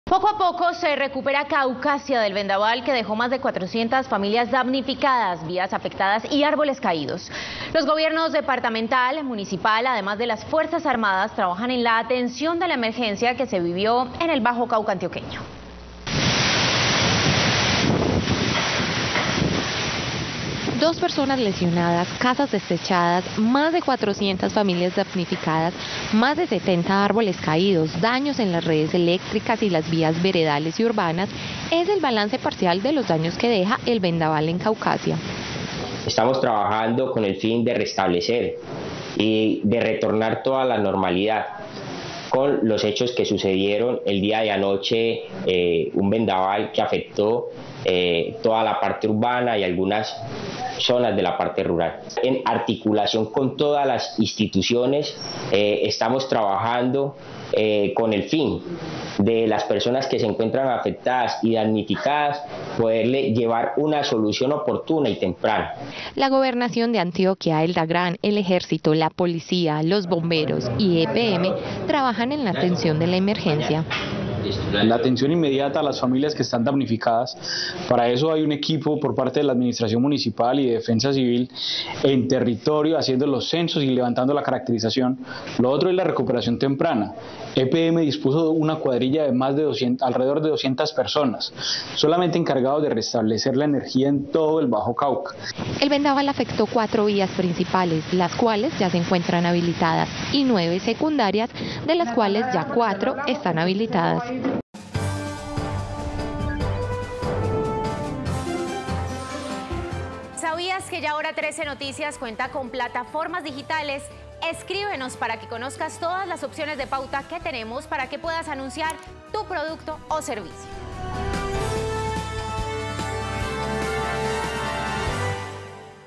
Vendaval en Caucasia deja más de 400 familias afectadas
Poco a poco se recupera Caucasia del vendaval ocurrido el día martes 26 de septiembre, el cual dejó más de 400 familias damnificadas, vías afectadas y árboles caídos.